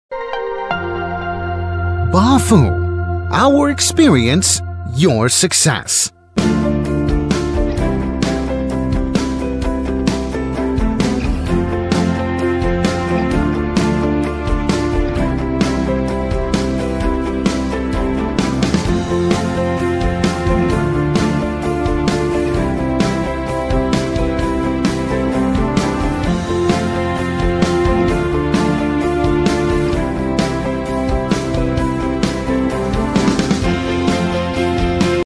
máquina de envasado de mascarillas, máquina de envasado de mascarillas faciales
BAFU FWL280 La máquina de paquete de flujo horizontal es aplicable al envasado de productos sólidos con forma regular, como galletas, galletas, chocolate, dulces de forma larga, pan, fideos instantáneos, hielo residual, helado, pastel de luna, medicina, productos, cuchillo y tenedor. Hardware, etc. La velocidad de embalaje hasta 350 unidades / min. Para obtener más información sobre el concepto de automatización de embalaje de Bafu, obtenga más información en:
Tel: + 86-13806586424